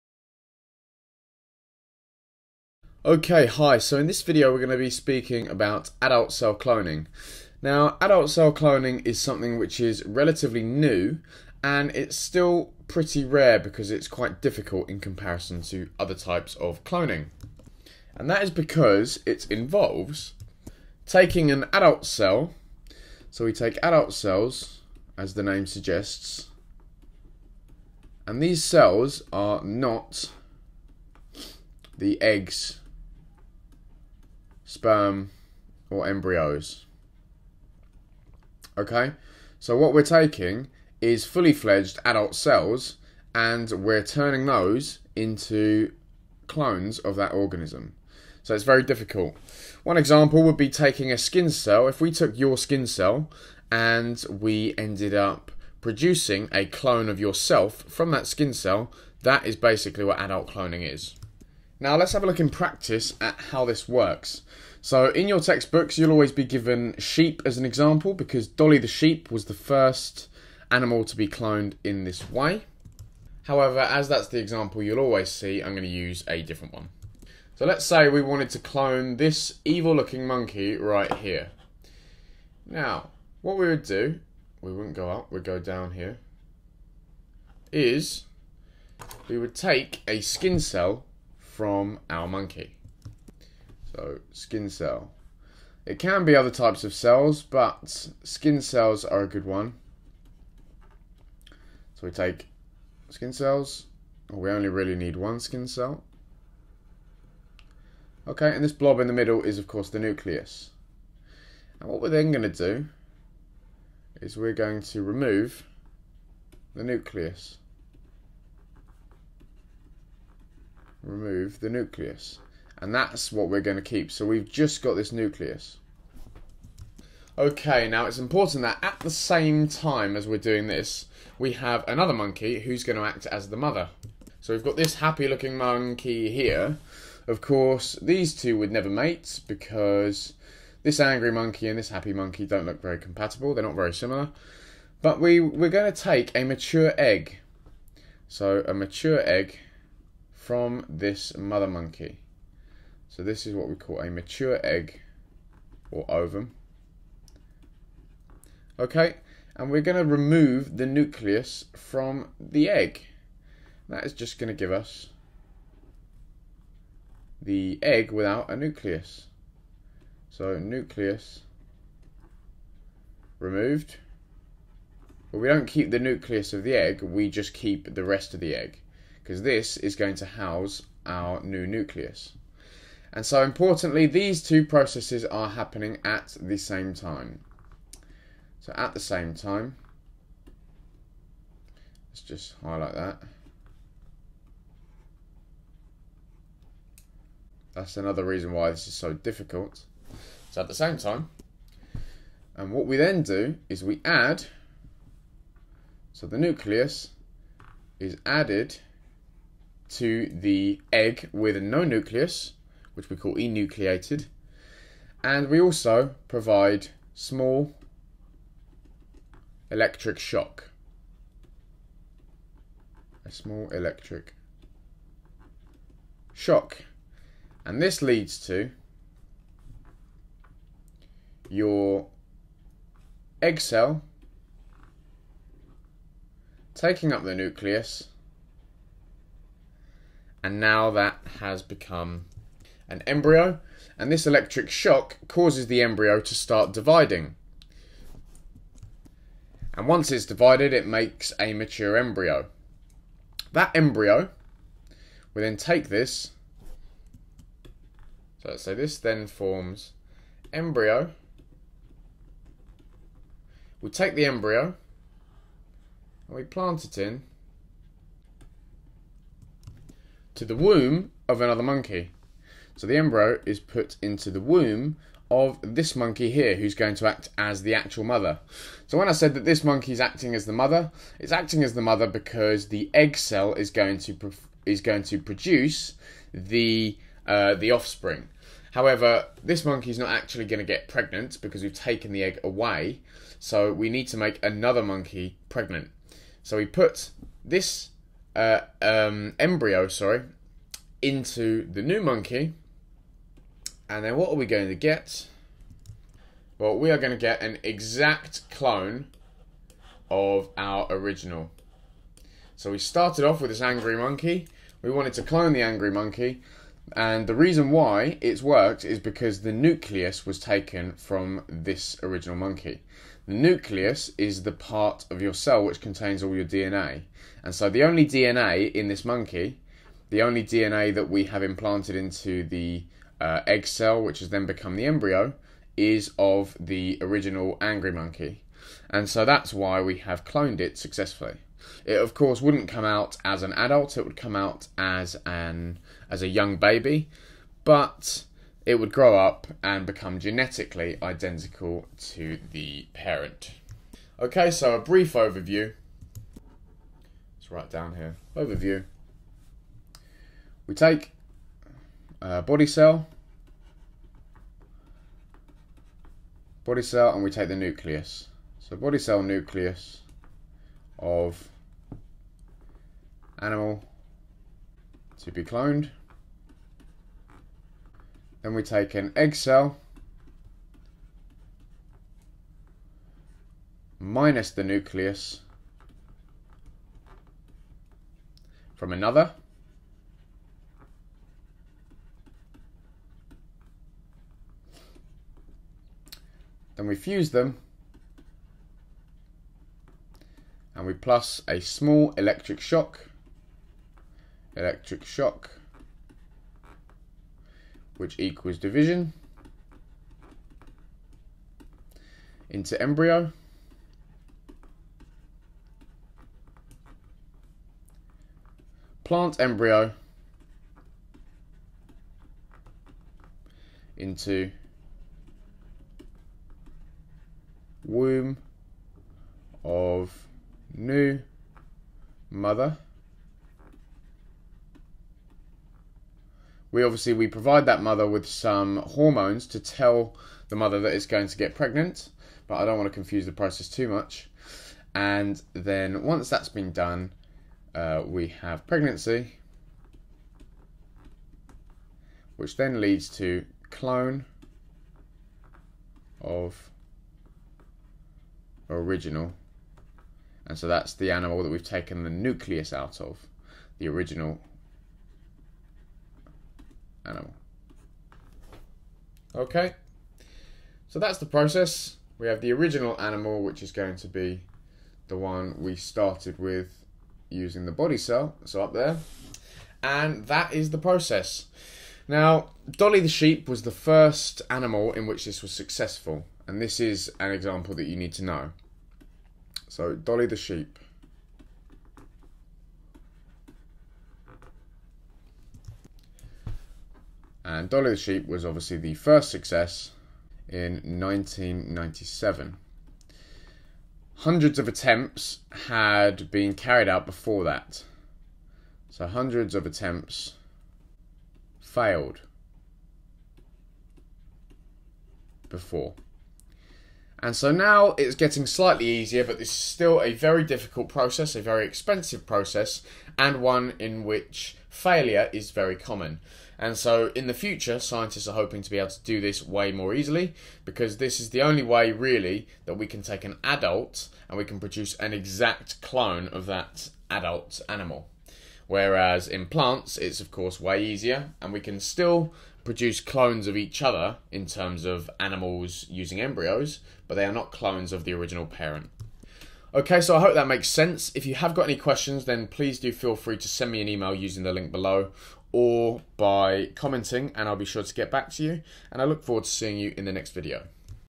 Adult Cell Cloning - AQA Biology
Having previously discussed the methods of cloning plants and animals, we now take a look at the most difficult yet the most intriguing form of cloning available to us. This allows us to produce absolute genetic replicas of healthy adults by taking some of their DNA.